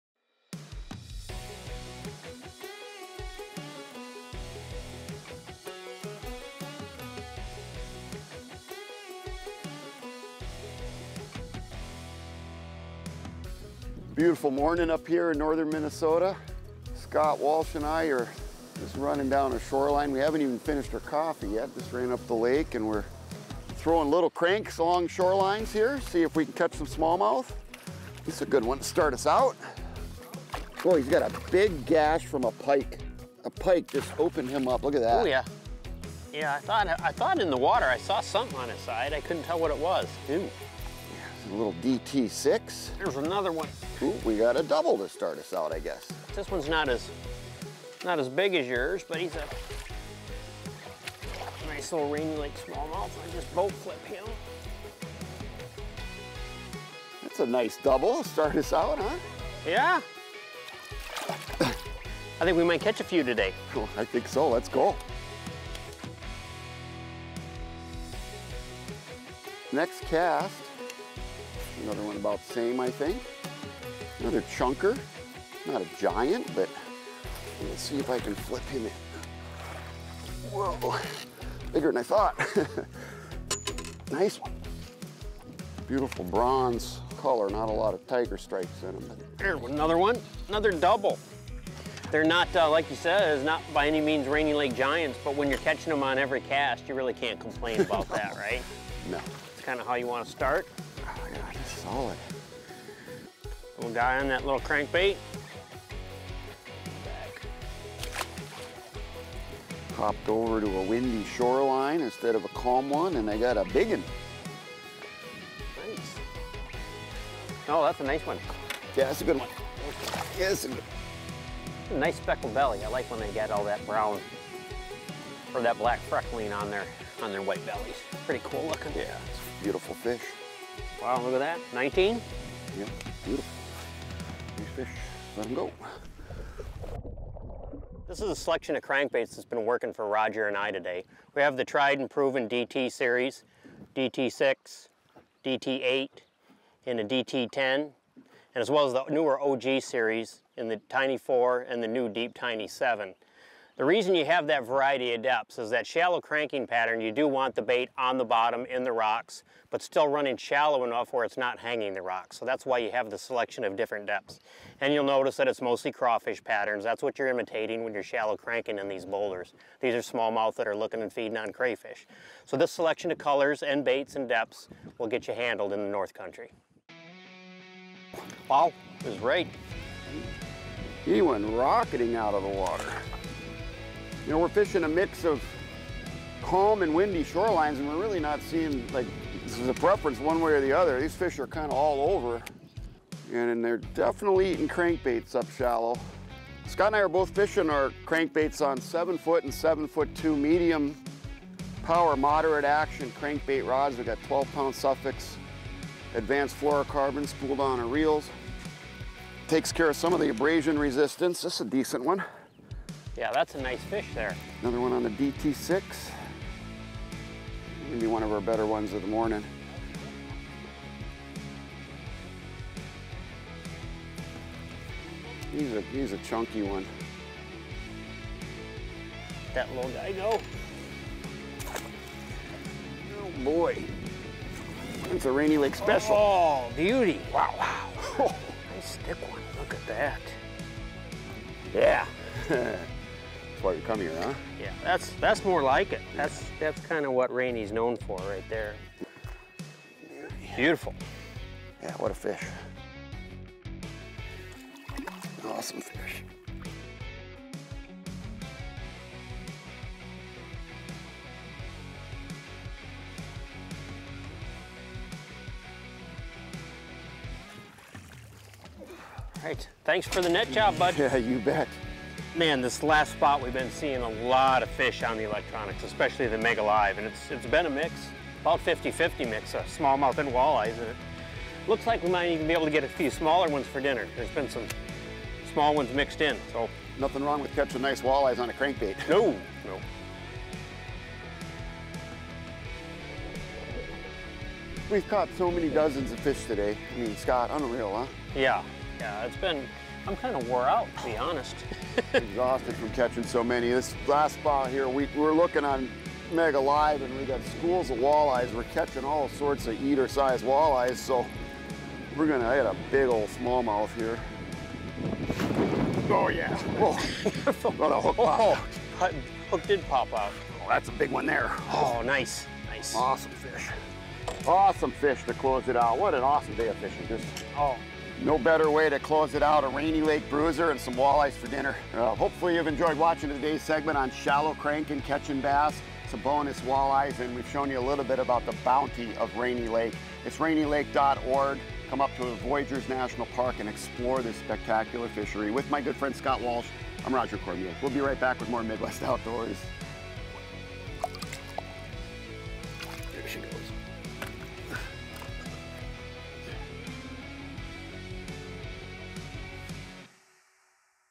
How To: Shallow running crankbaits for more bass!
Roger Cormier and Scott Walsh are on Rainy Lake targeting smallmouth using shallow running crankbaits.